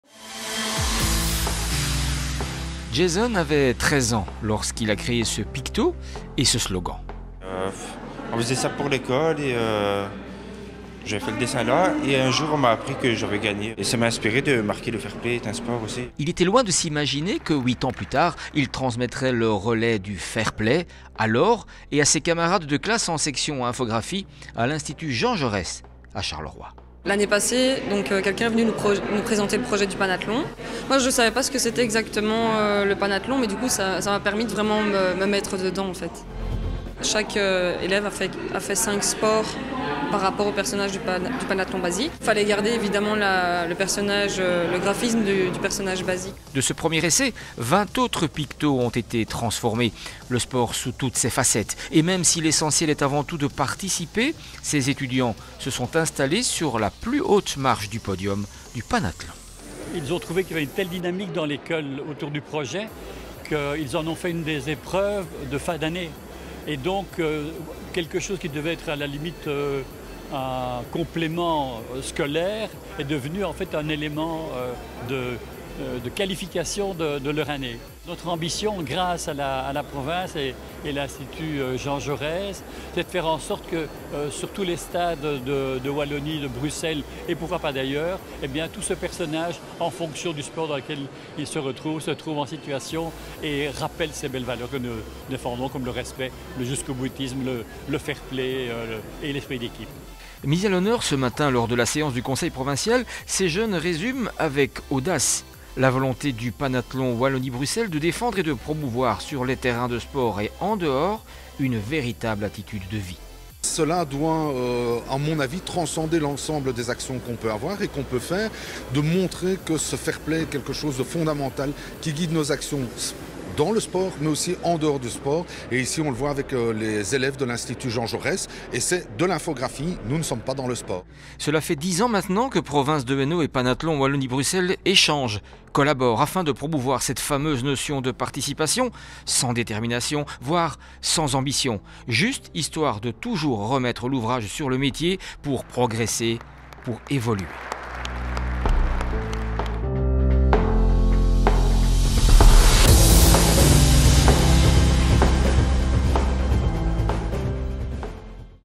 L’Institut Jean Jaurès décroche la palme !
(FAIR-PLAY)
"Le fair-play est un sport".
Pour faire résonner ce slogan et l’accompagner d’un visuel, le Panathlon Wallonie-Bruxelles avait lancé un vaste appel à projet auprès des écoles francophones. Ce sont les élèves de l’Institut Jean Jaurès de Charleroi qui se sont révélé les plus créatifs, démontrant une nouvelle fois le dynamisme de l’enseignement provincial. Le Conseil provincial de ce 25 mai a reçu ce petit groupe de férus en publicité et infographie pour saluer leur performance.